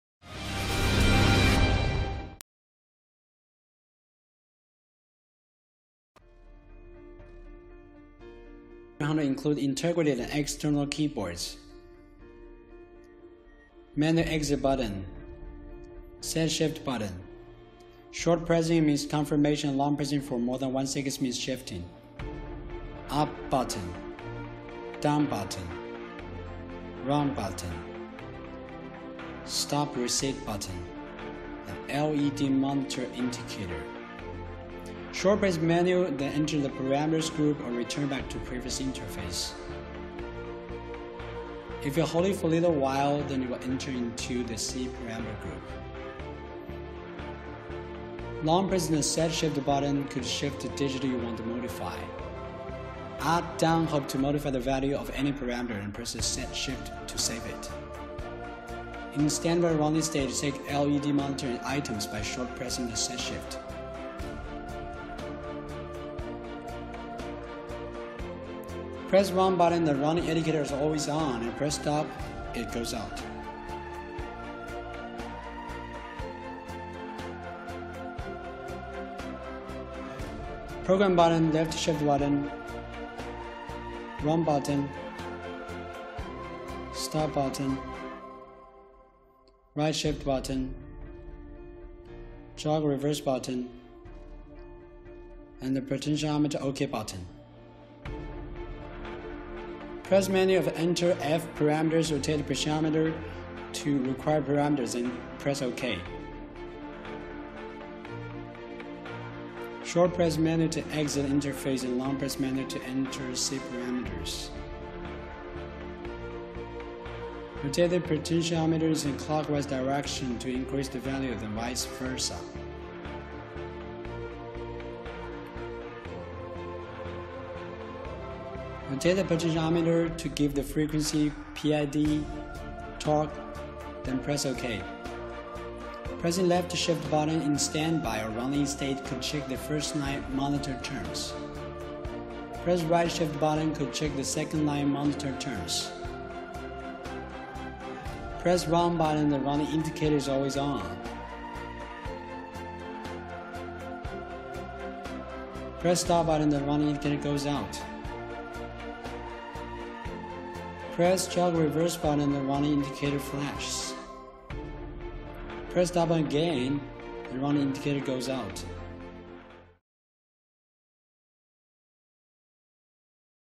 VEICHI AC300 Keyboard Function
A brief introduction of AC300 keyboard buttons, functions and demonstration.